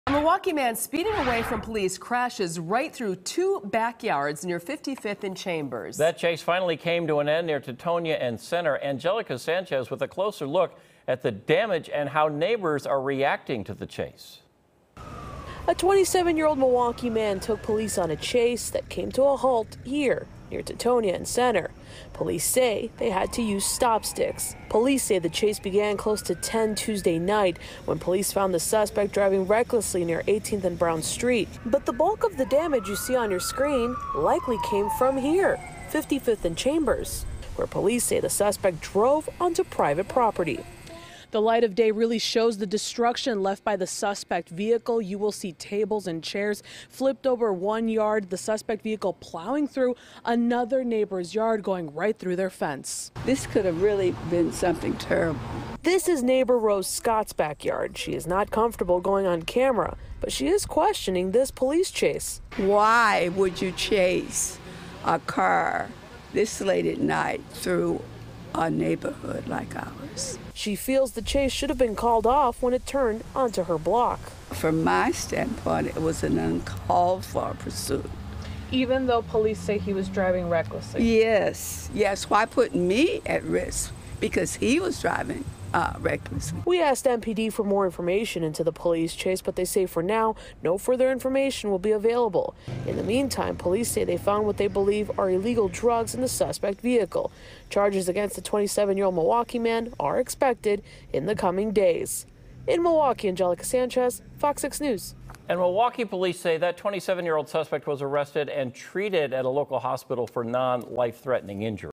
Teutonia and Hadley police chase, crash | FOX6 News Milwaukee
A Milwaukee man speeding away from police crashed through two backyards near 55th and Chambers before the chase finally came to an end near Teutonia and Center.